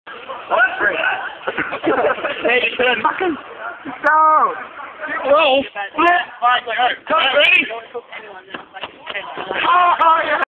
Max's Frontflip
Max doing a frontflip coz Mikey didnt have the balls to do it...